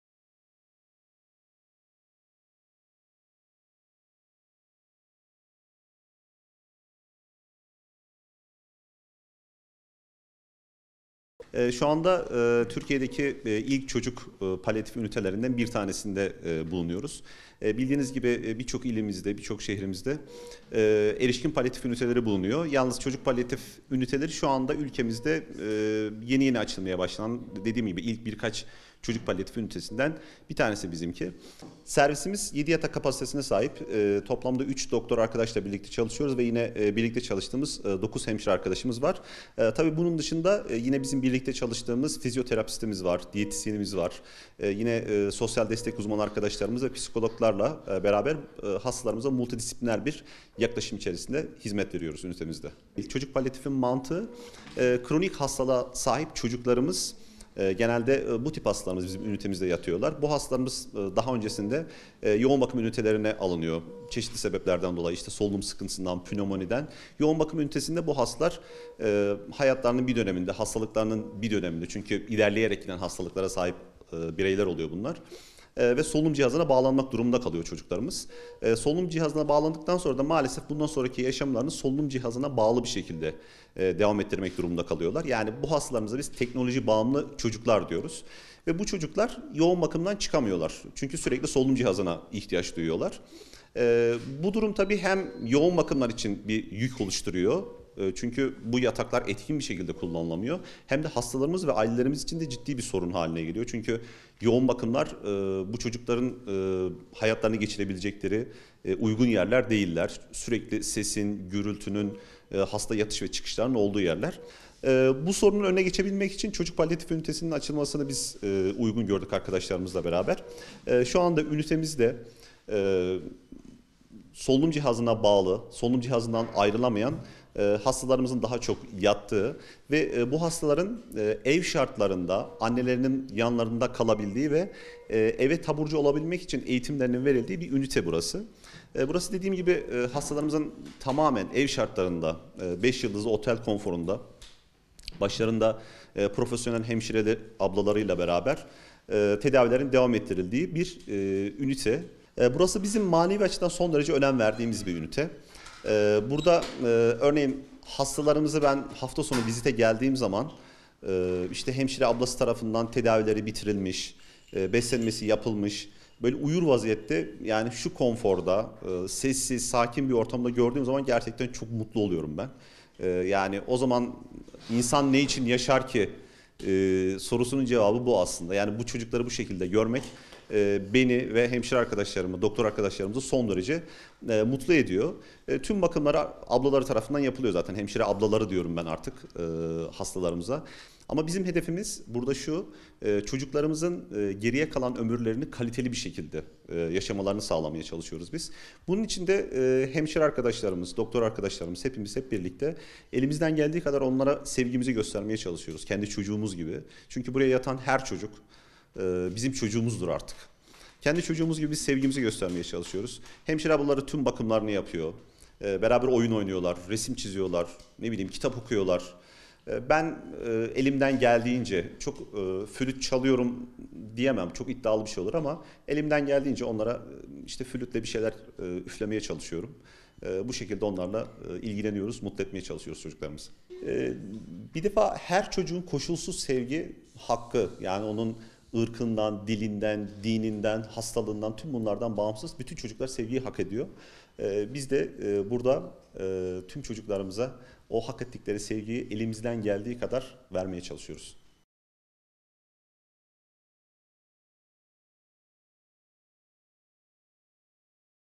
Çocukları hem tedavi ediyor hem flüt çalarak onlara moral veriyor
◥Kayseri Şehir Hastanesi'nde Çocuk Palyatif Bakım Ünitesinde görevli 13 yıllık Uzman Doktor Adem Dursun (38), çocukları hem tedavi ediyor hem de yan flüt çalarak moral veriyor. Dursun, "Bizim amacımız çocuklarımızın geriye kalan ömürlerini kaliteli bir şekilde geçirmelerini sağlamak. Ben elimden geldiğince flüt çalıyorum. Sevgi, her çocuğun koşulsuz hakkıdır" dedi.

◥Kayseri Şehir Hastanesi'nde 3 yıl önce kurulan Çocuk Palyatif Merkezi'nde, 3 doktor ve 9 hemşire görev yapıyor. Serviste, Spinal Müsküler Atrofi (SMA) başta olmak üzere Serebral Palsi (SP) ve kronik hastalığı bulunan çocuklar tedavi görüyor. Çocukların tüm kişisel bakımları, yardımcı bakım elemanları ve hemşireler tarafından yapılıyor. Serviste görevli hemşireler çocuklarla oyunlar oynayıp boyama yaparken, çocuk yoğun bakım servisinde görevli Uzman Dr. Adem Dursun ise, tedavi ettiği çocuklara yan flüt çalarak moral veriyor.

◥Doktor Dursun, çocuk palyatif ünitelerinin yeni açılmaya başladığını söyleyerek, "Kronik hastalığa sahip çocuklarımız burada tedavi görüyor. Bu tip hastalar yoğun bakım ünitelerinde tedavi görüyordu. Bu çocukların hastalıkları ilerleyerek gidiyor ve solunum cihazına ihtiyaç duyuyorlar. Aileler ve hastalar için uygun ortam oluşmayınca talep üzerine çocuk palyatif ünitesinin açılması uygun görüldü. Ünitemizde solunum cihazından ayrılamayan çocuklar, ev şartlarında anneleri yanında kalacak şekilde 5 yıldızlı otel konforunda tedavi ediliyor" diye konuştu.

◥Doktor Dursun, çocuk palyatif servisine manevi açıdan çok önem verdiklerini belirterek, "Profesyonel hemşireler tarafından bakılan çocuklarımız ile aramızda güçlü bir bağ var. Hafta sonu vizite geldiğim zaman çocuklarımızın huzurlu ve sessiz ortamda olmaları beni çok mutlu ediyor. Anne ve babası yanındayken çocuk hiçbir tepki vermezken doktor ya da hemşireleri görünce çocuk mutlulukla ona atılıyor. Bunlar kronik hastalığı olan çocuklar. Hiçbiri bu hastalığı kendi seçmedi. Bizim amacımız, çocuklarımızın geriye kalan ömürlerini kaliteli bir şekilde geçirmelerini sağlamak. Bunun için hemşire arkadaşlarımız çocuklarla kitap okuyor, boyamaya yapıyor. Ben elimden geldiğince flüt çalıyorum. Sevgi, her çocuğun koşulsuz hakkıdır. Biz de çocuklarımıza hak ettikleri sevgiyi elimizden geldiği kadar vermeye çalışıyoruz" ifadelerini kullandı.